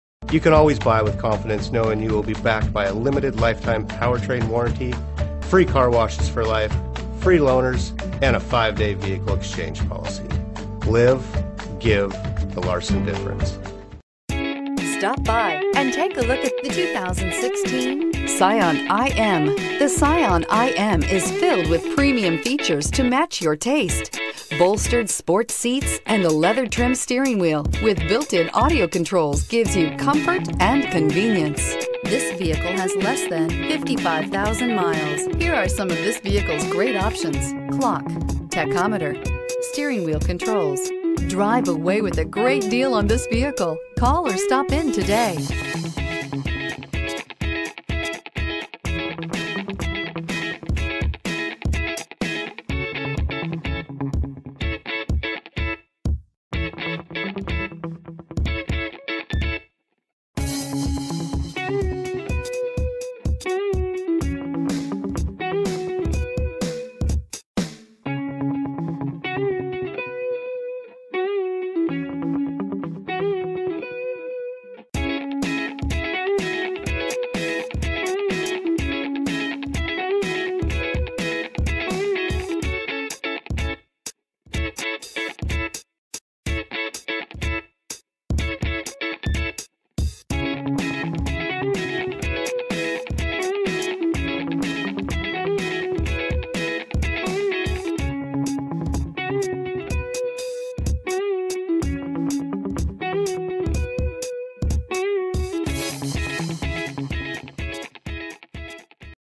2016 Scion iM HP2105
2016 Scion iM Base

Larson Hyundai
7601 S. Tacoma Way

Classic Silver Metallic 2016 Scion iM FWD CVT 1.8L 4-Cylinder DOHC, with Black Cloth Interior.